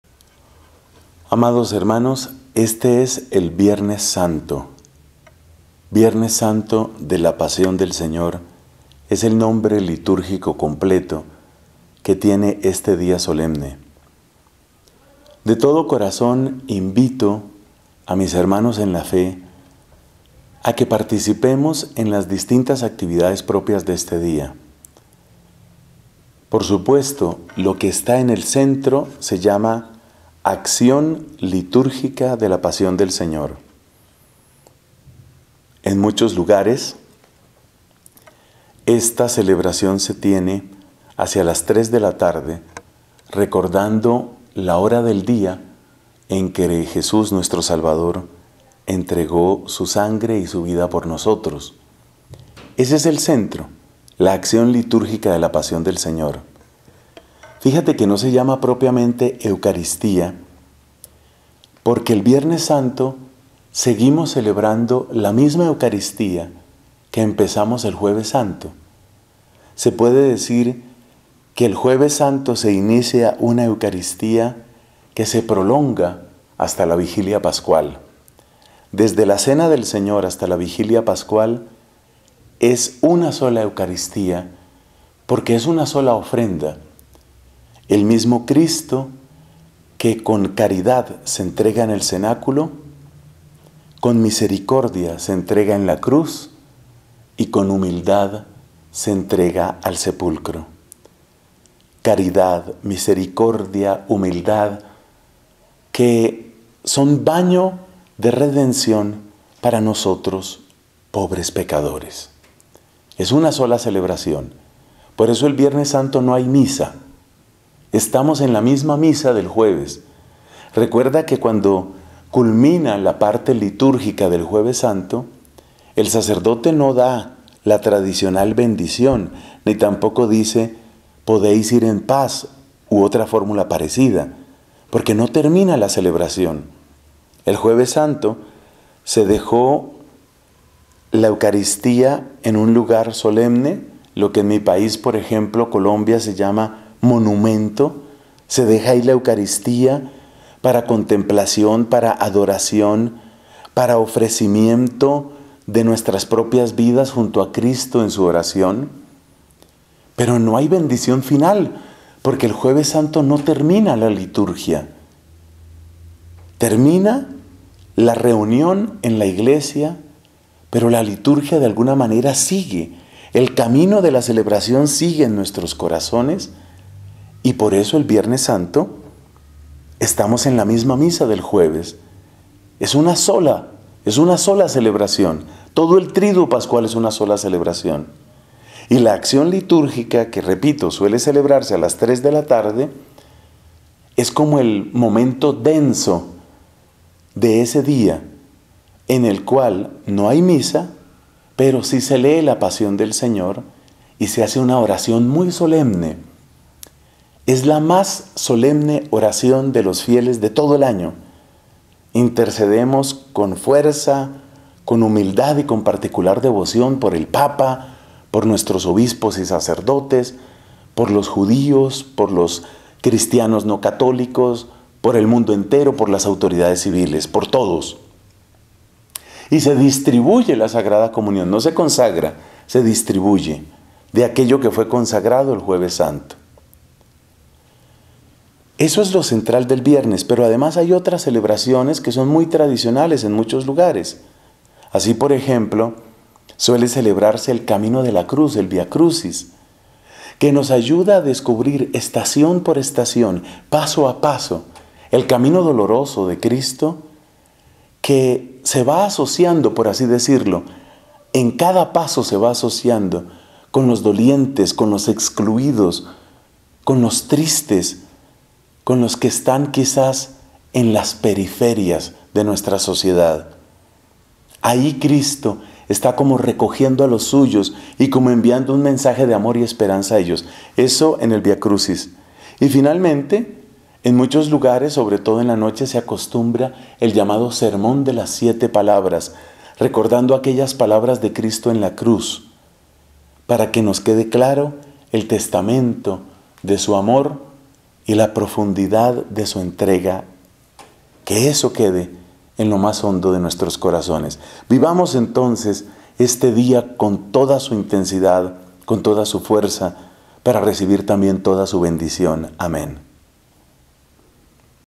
LA GRACIA del Viernes 25 de Marzo de 2016
VIERNES SANTO DE LA PASIÓN DEL SEÑOR

Celebremos con gran solemnidad la caridad con que Cristo se entrega en el cenáculo, su misericordia en la cruz y su humildad en el sepulcro.